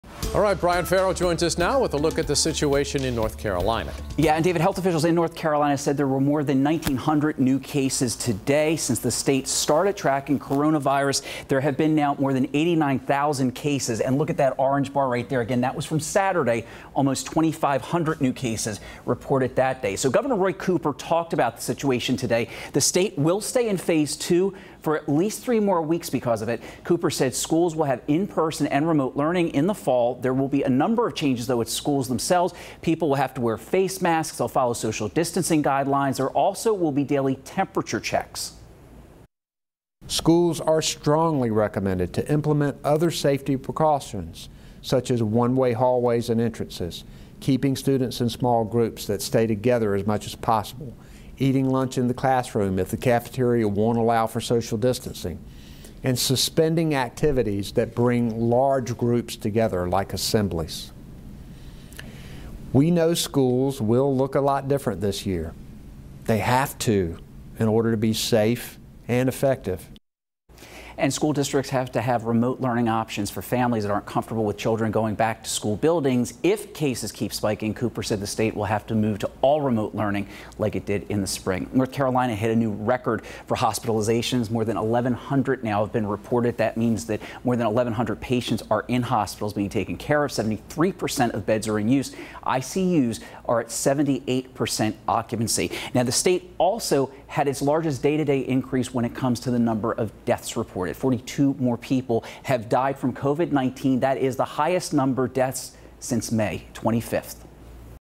North Carolina Governor Cooper provides guidance on school reopenings
The governor said that North Carolina schools will be open this fall for remote learning and in-person instruction.